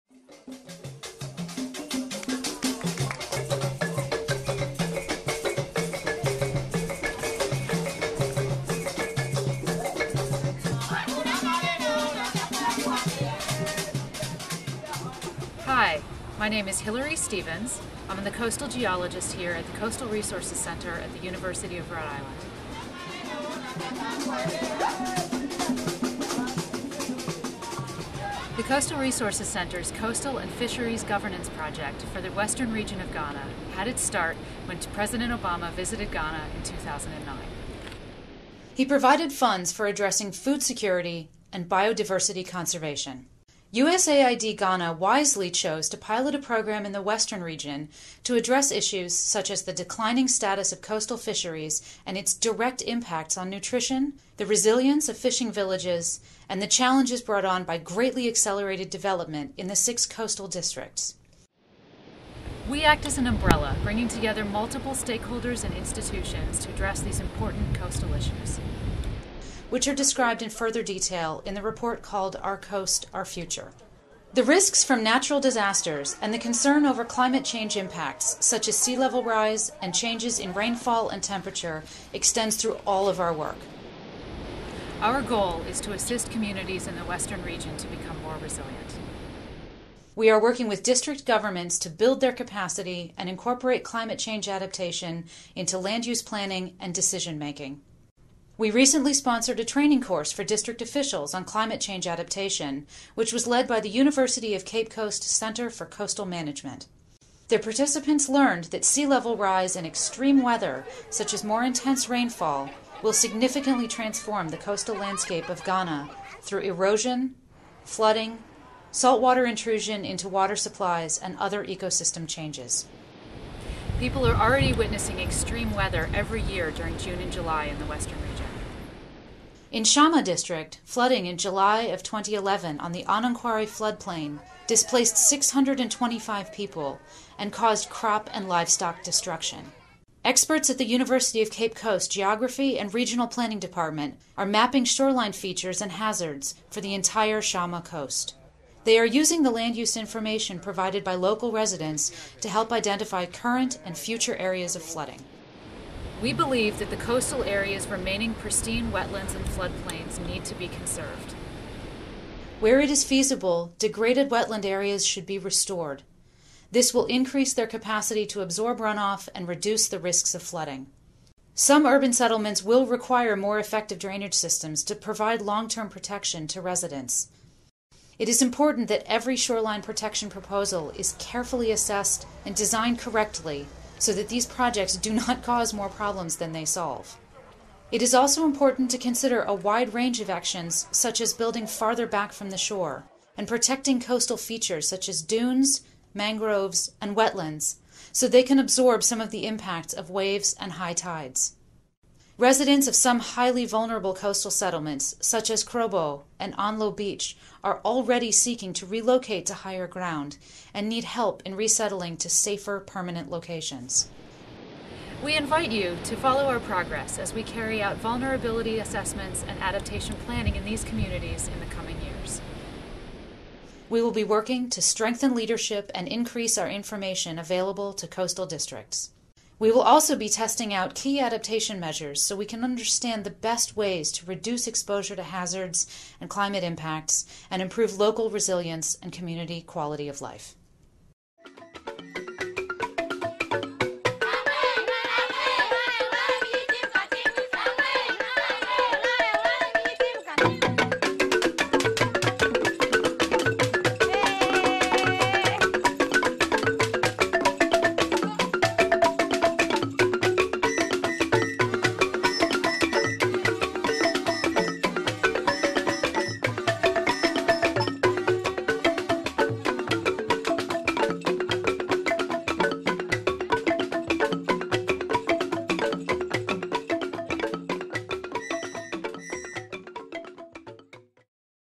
Climate Change Adaptation for the Coastal Communities of Ghana's Western Region
The Hen Mpoano (Our Coast) Project is carried out by the Coastal Resources Center in collaboration with Friends of the Nation, The WorldFish Center and Sustainametrix, to help coastal communities in the Western Region of Ghana and its six coastal districts assess climate change impacts and community vulnerability in order to find ways to adapt using their own resources and knowledge.

The Coastal Resources Center collaborates with USAID and other partners to carry out natural hazard and climate change vulnerability assessments, put adaptation measures into place and share lessons about what works and what needs to be done.

This is one of a series of short videos exploring practical approaches being used right now by our colleagues in several different coastal countries to manage risk and take no-regrets actions to increase their resilience and adapt to the impacts of climate change.

We would like to acknowledge and thank Skyy TV Takoradi for video clips of the 2011 flooding in Shama District.

Links:

Enhancing climate adaptation through land use planning in Shama District, Ghana

The Integrated Coastal and Fisheries Governance (ICFG) Initiative for the Western Region of Ghana

Friends of the Nation

Adapting to Coastal Climate Change: A Guidebook for Development Planners